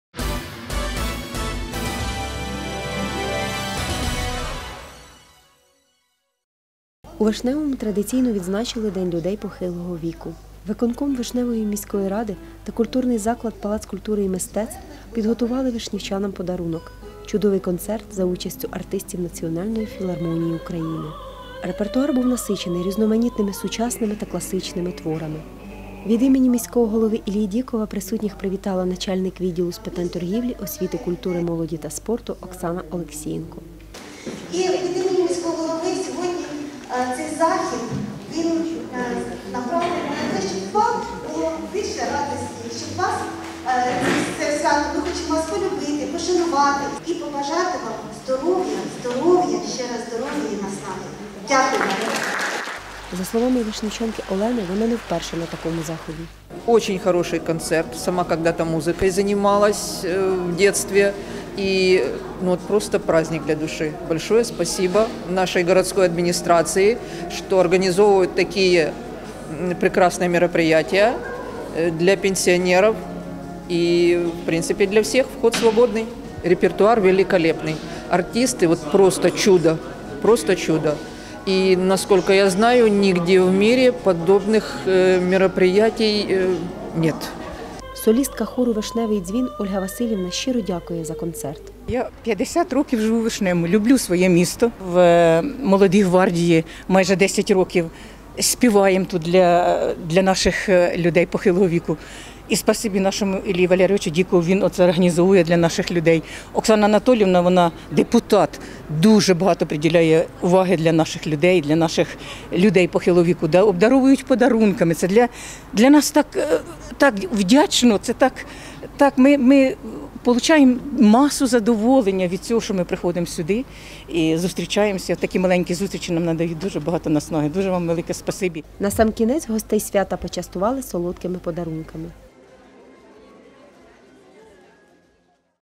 У Вишневому відзначили День людей похилого віку
КЗ «Палац культури і мистецтв» та виконком Вишневої міської ради  підготували вишнівчанам подарунок – чудовий концерт артистів Національної філармонії України. Перед присутніми виступив квартет музикантів «Київ», а також  солісти філармонії, заслужені артисти України. Репертуар був насичений різноманітними творами, як класичними, так і  українських композиторів
  Начальник  відділу з питань торгівлі, освіти, культури Оксана Олексієнко тепло привітала  присутніх від імені міського голови іллі Дікова.